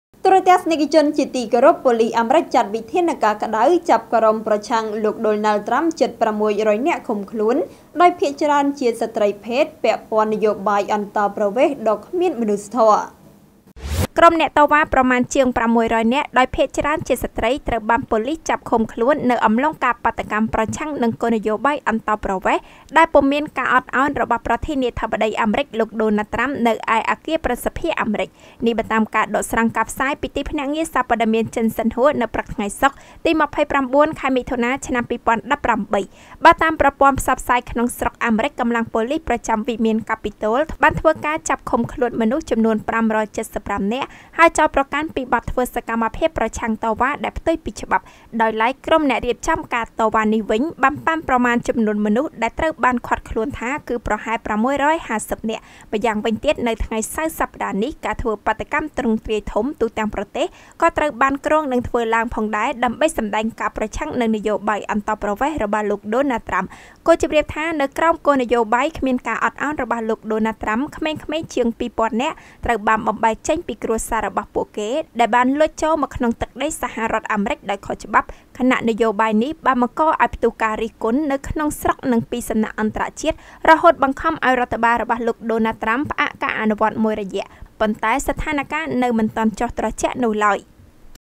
​ប៉ូលិស​អាមេរិក​ចាត់វិធានការក្តៅ «ចាប់ក្រុមប្រឆាំង ដូណាល់ ត្រាំ ជិត៦០០នាក់ឃុំ...
​ប៉ូលិស​អាមេរិក​ចាត់វិធានការក្តៅ «ចាប់ក្រុមប្រឆាំង ដូណាល់ ត្រាំ ជិត៦០០នាក់ឃុំខ្លួន» ដោយភាគ​ច្រើនជាស្ត្រីភេទ ពាក់ព័ន្ធនយោបាយអន្តោប្រវេសន៍ដ៏គ្មានមនុស្សធម៌! (រាយការណ៍ព័ត៌មានដោយកញ្ញា សំ ស្រីនីត​ និងកញ្ញា​ យ៉ង់ សក្គុណា​, Fresh News TV)